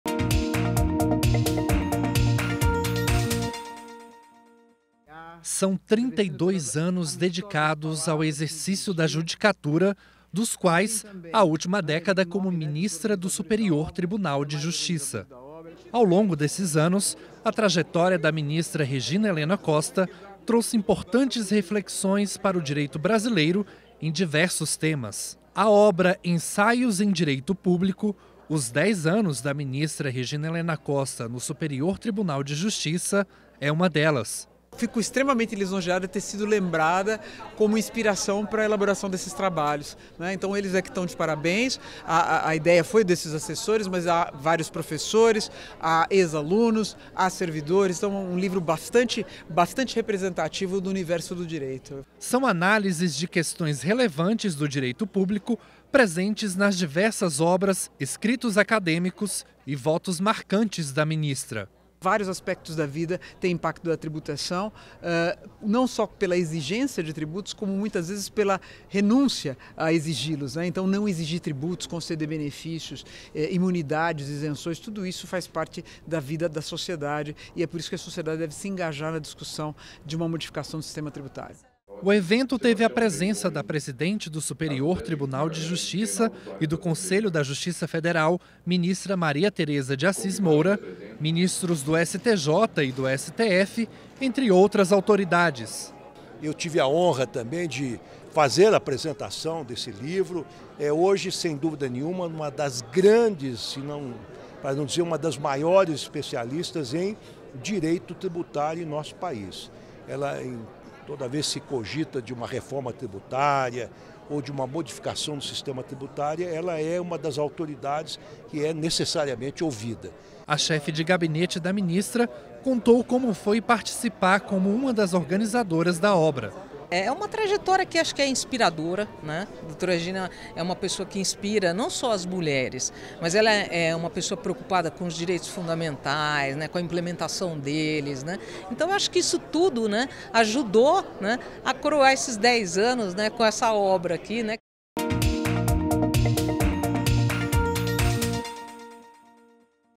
Temas atuais do direito público são destaque de obra em homenagem à ministra Regina Helena Costa
A poucos dias de completar uma década de atuação no Superior Tribunal de Justiça (STJ) – sua posse ocorreu em 28 de agosto de 2013 –, a ministra Regina Helena Costa foi homenageada na noite desta quarta-feira (23) com o lançamento de uma coletânea com 41 artigos, de 63 autores, sobre algumas das discussões mais palpitantes do direito tributário e de outras áreas do direito público. O evento aconteceu no Espaço Cultural STJ.​​​​​​​​​
O livro Ensaios em Direito Público – Os dez anos da ministra Regina Helena Costa no Superior Tribunal de Justiça foi organizado pela chefe de gabinete da ministra, Marilia Carvalho Neves Ferros; o juiz auxiliar Fernando Caldas Bivar Neto; o assessor da magistrada Samuel Rodrigues de Miranda Neto; e o jurista Lázaro Reis Pinheiro Silva. A apresentação da obra coube ao ministro aposentado do Supremo Tribunal Federal (STF) Ricardo Lewandowski.